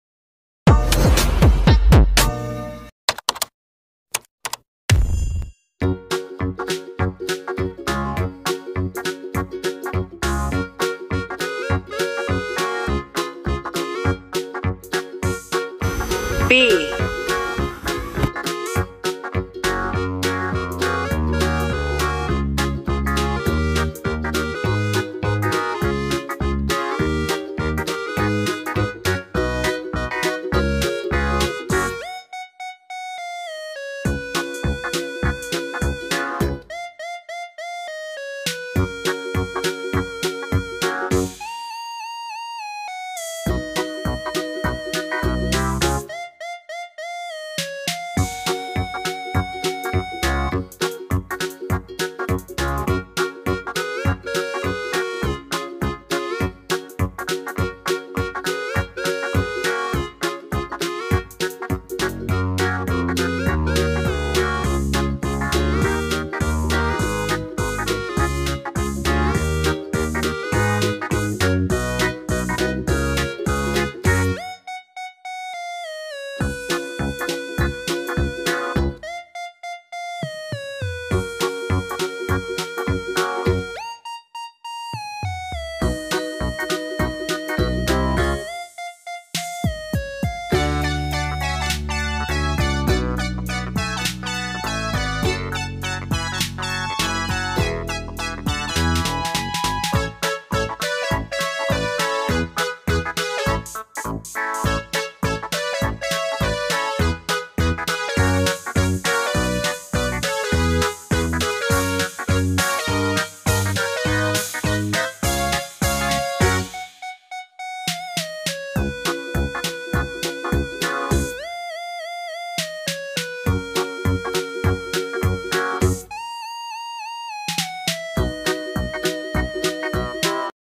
Draw a butterfly from the letter B, Easy drawing for kids.
Welcome to Art4 Kids 🤗🤗🌈🌈
How to draw and color a butterfly  Draw easy and fun coloring for kids.